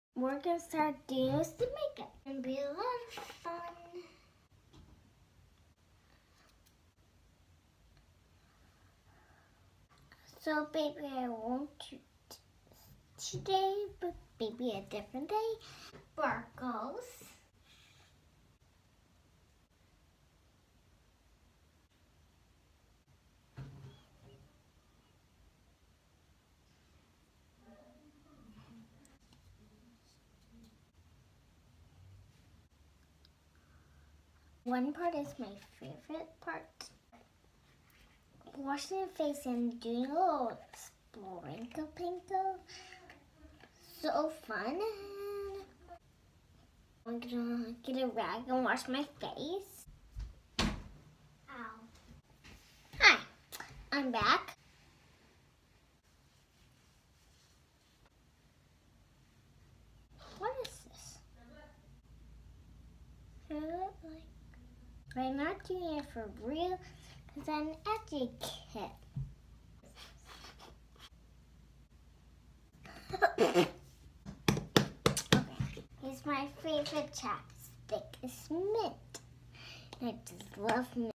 Fellowship Ranch: makeup, the Emi way...
Today on Fellowship Ranch, the BEST makeup tutorial on planet earth;)